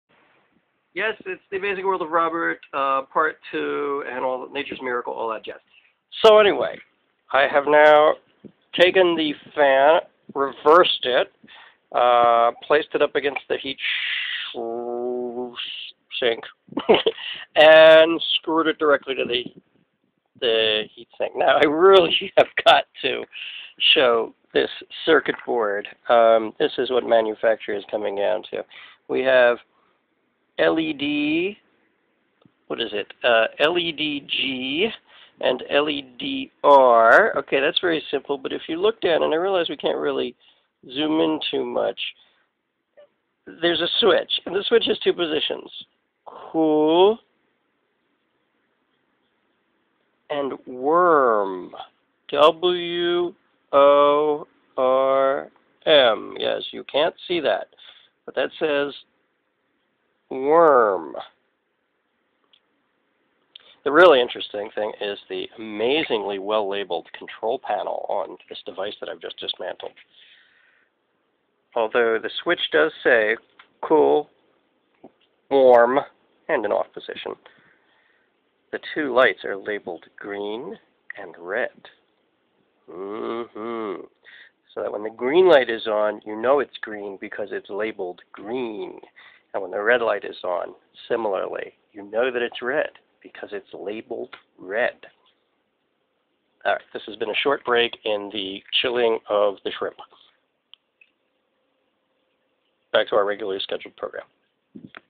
My Dumb Projects: The Fish Tank Chiller, Part B
A closer look at the technological heart of the device.